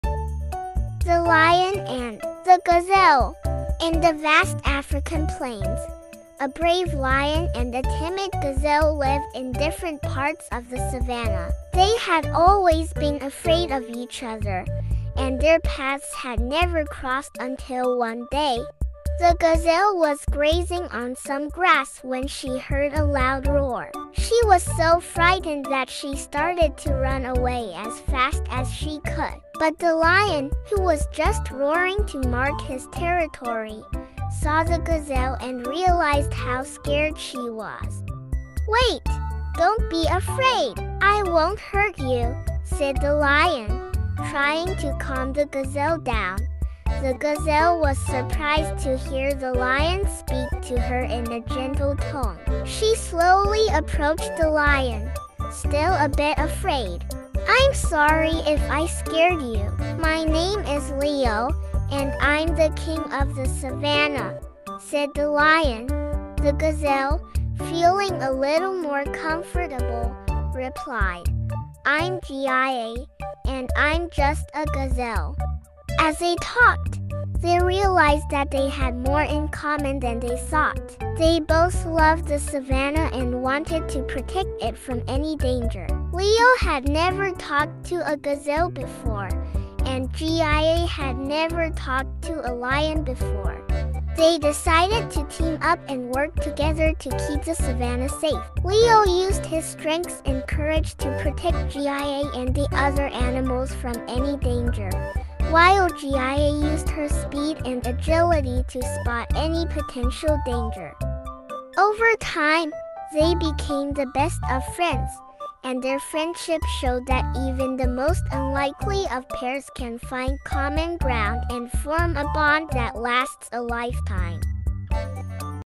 The Lion and the Gazelle | short story in English for kids | Short Stories
Leo the lion and Gia the gazelle live in the African savannah and have always been afraid of each other. But one day, they realize they have more in common than they thought and decide to team up to protect the savannah. Their friendship grows stronger as they work together, showing that even the most unlikely of pairs can form a bond that lasts a lifetime.